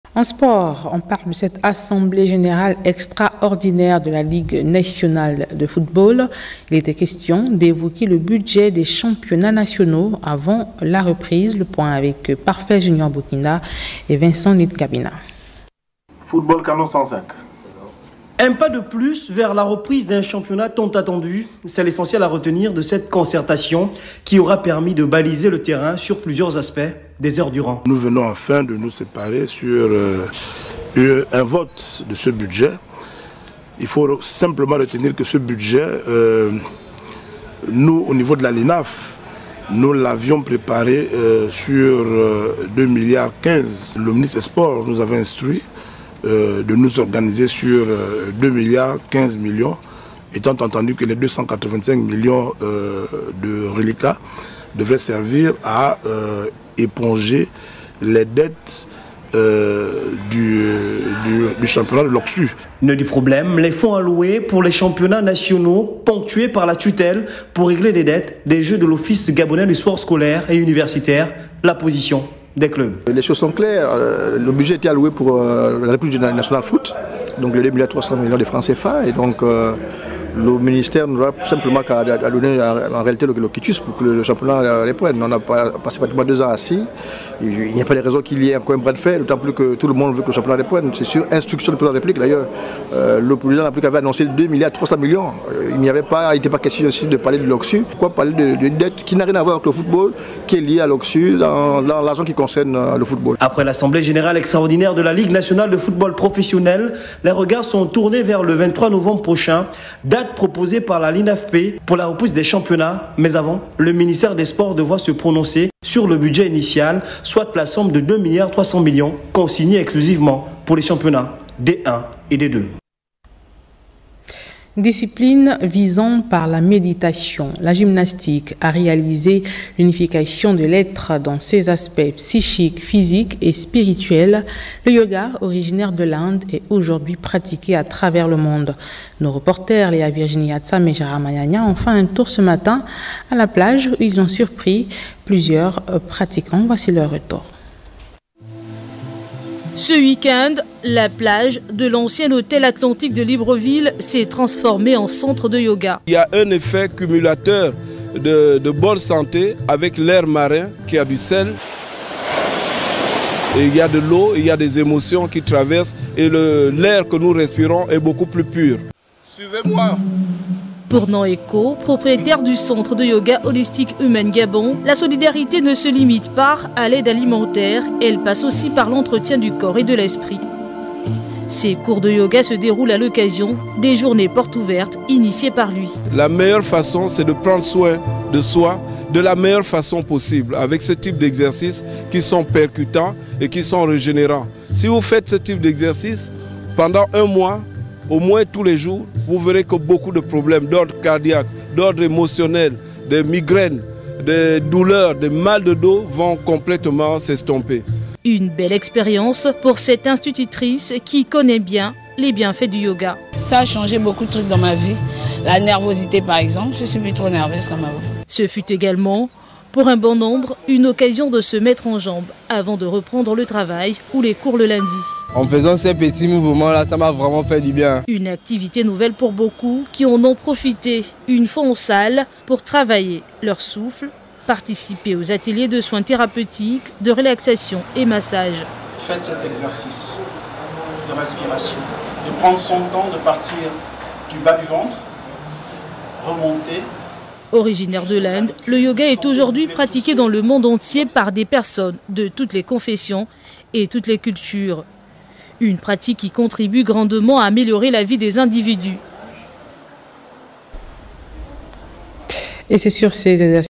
INSTANTS SPORTS FOOTBALL ET YOGA
* Assemblée générale de la ligue nationale de football en vue de préparer l'ouverture du championnat prévue au mois de novembre 2024
* La pratique du yoga et ses bienfaits